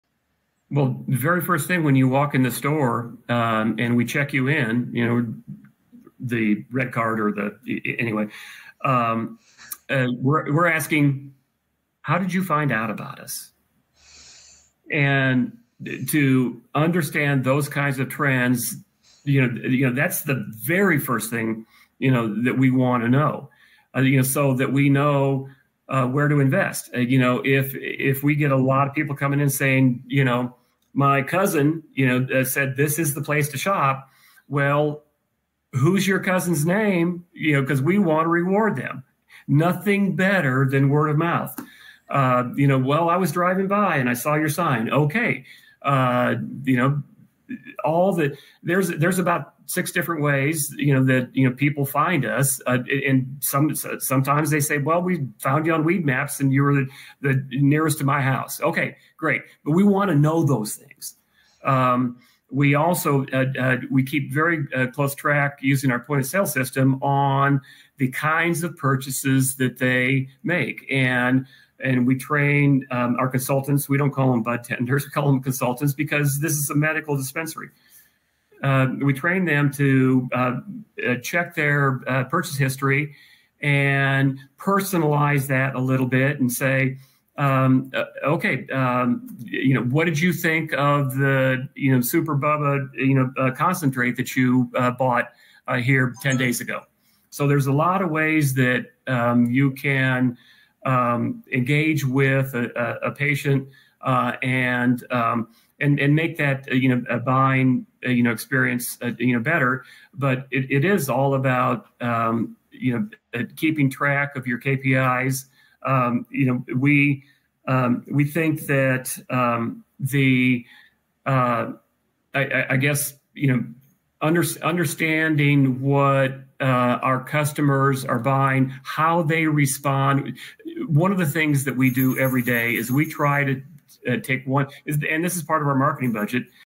Boost Your Sales with These Expert Tips From a Dispensary Owner
Learn the secrets to optimizing your dispensary's sales with proven strategies. Discover how to track customer trends, personalize the buying experience, and maximize your marketing budget. Improve customer satisfaction and increase revenue.